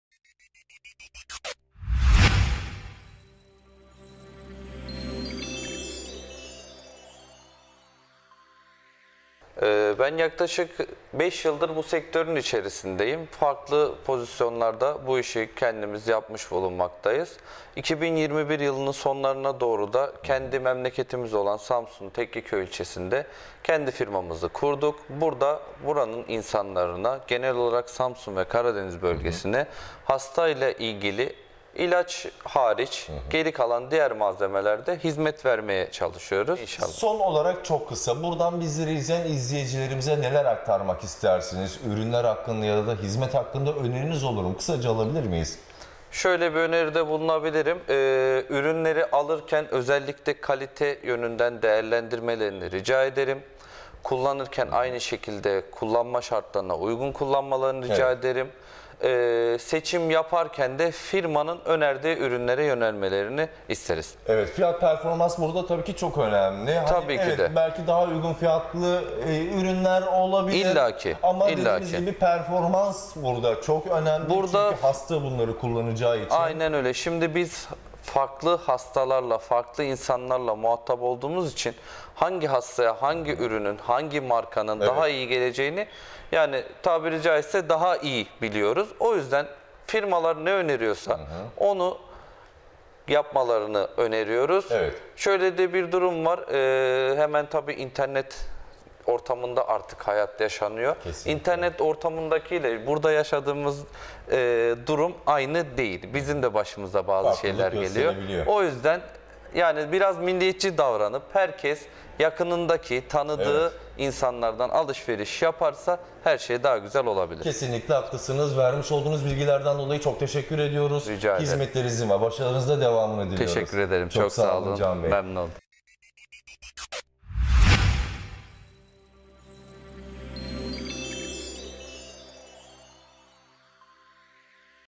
TEK MEDİKAL - Tekkeköy / SAMSUN
TEK MEDİKAL firma sahibi Hasan Can SOYKAN, başarı ve faaliyetlerini Karadeniz Haber TV muhabirimiz Cemal YERGİN' e anlattı.

Karadeniz’ in her yerinden sektör ve firma haberlerini bir arada toplayan,
Karadeniz' in özel sektör programı.